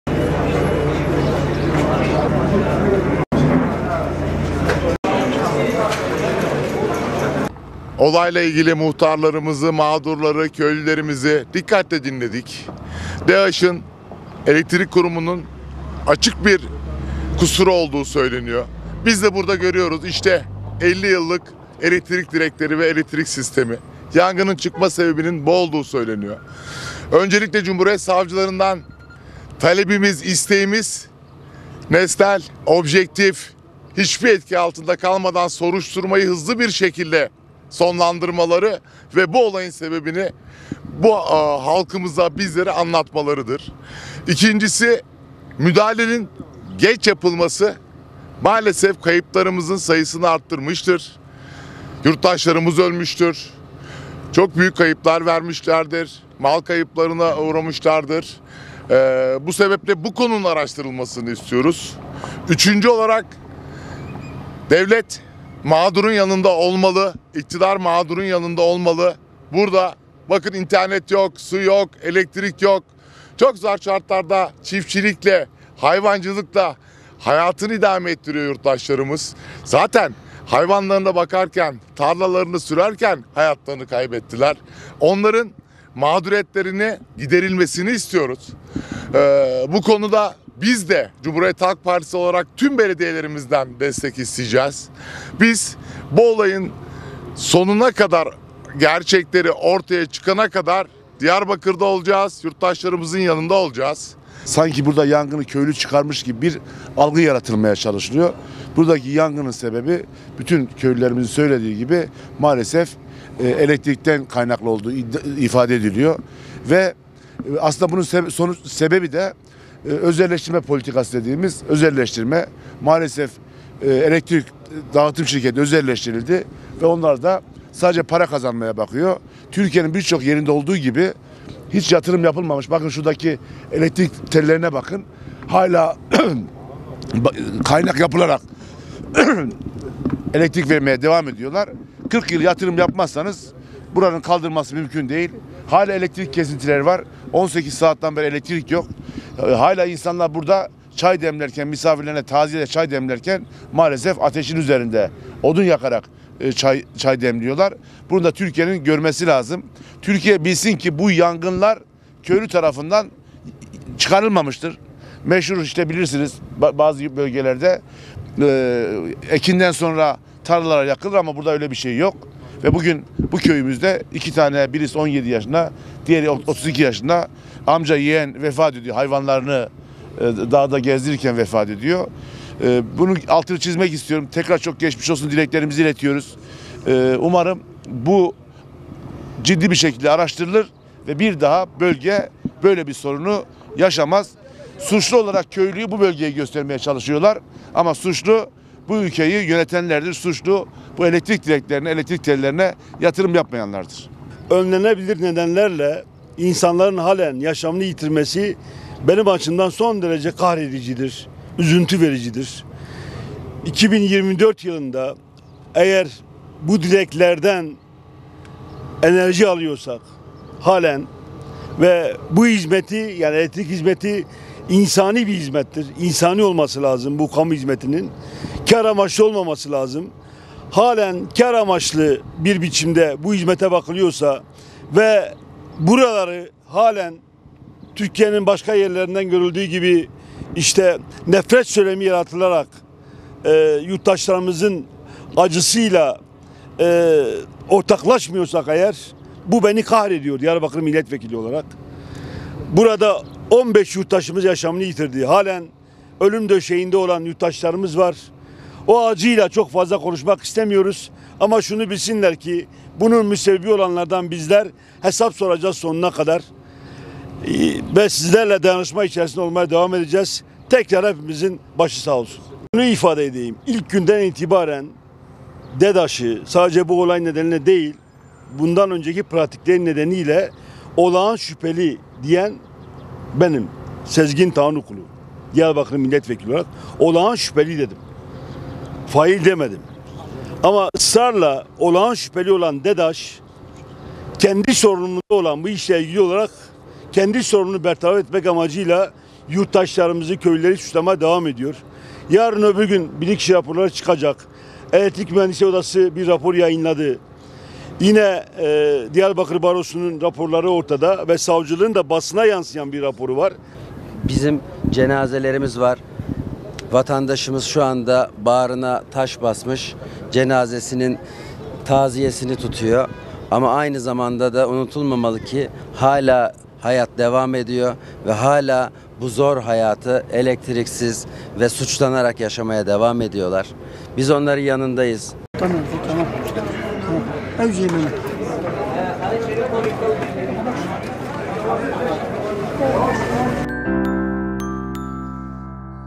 CHP HEYETİ YANGIN BÖLGESİNDE: SAVCILAR GÖREVE ÇAĞRILDI
Amed ve Mêrdîn arasındaki yangın bölgelerini ziyaret eden CHP heyeti, “Vatandaşımızın burada yaşamak ve burada doğmak dışında suçu yok. Bu suçsa evet suçluyuz” dedi.